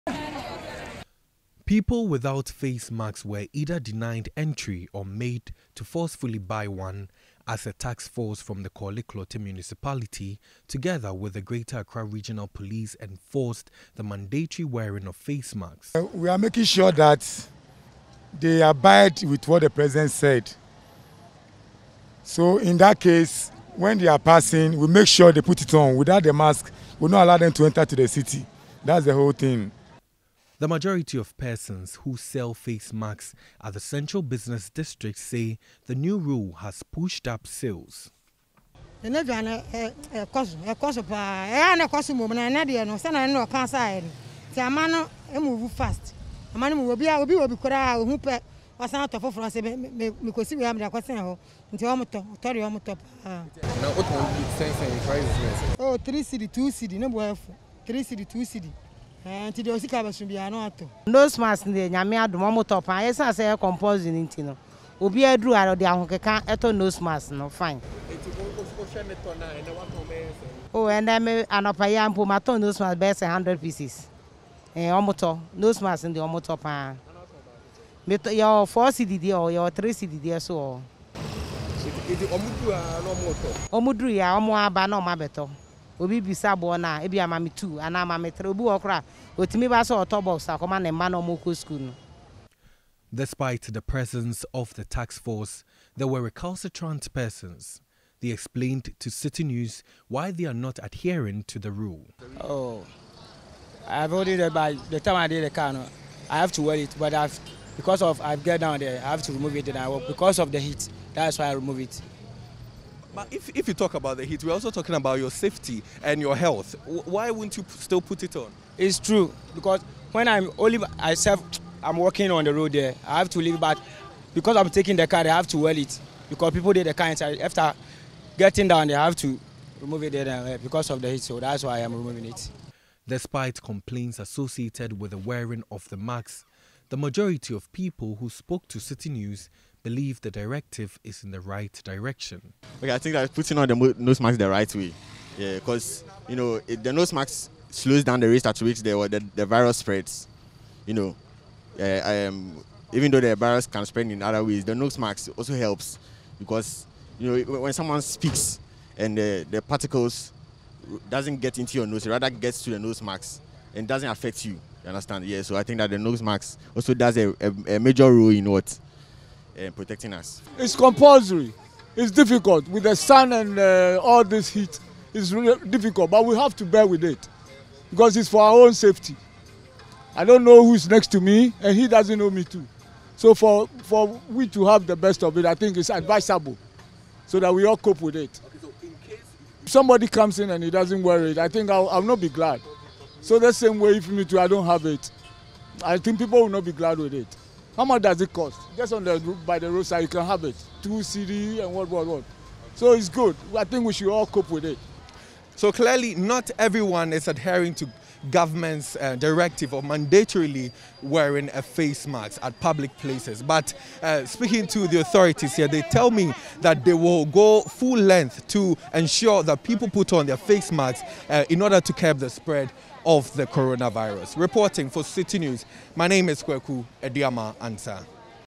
Local authorities begin enforcement of directive on mandatory wearing of face masks | Citi Newsroom
Local authorities in Accra have started enforcing the president's directive on the wearing of face masks.

In his 11th address to the nation on COVID-19, President Akufo-Addo said that it is now compulsory for every individual to wear a face mask when they step out of their homes.